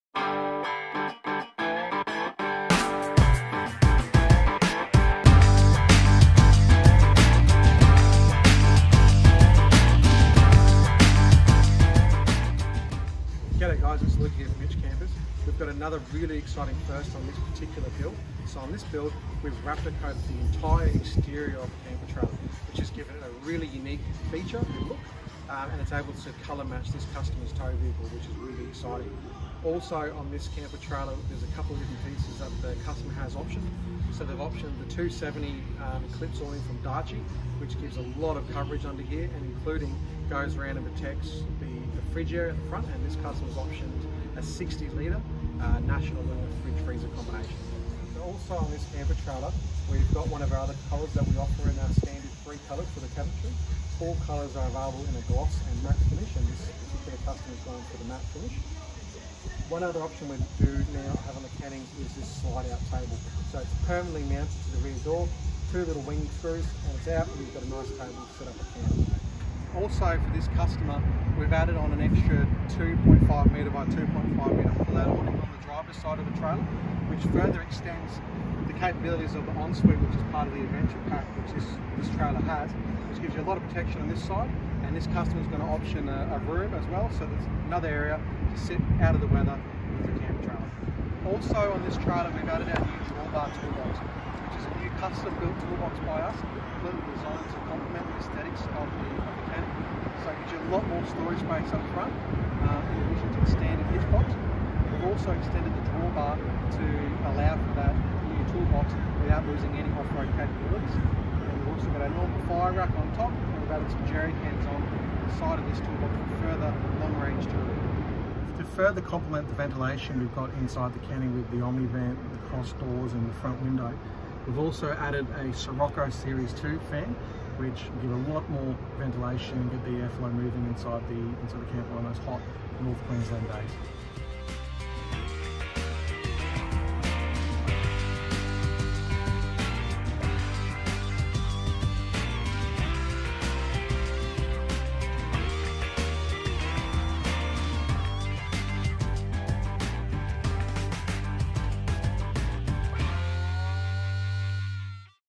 Raptor Coated Canning
Luke from Hitch Campers, takes you on a walkthrough on one of the latest builds to come out of Hitch Campers HQ. Some exciting new options are featured on this build and are avaliable on all 2021 Canning's.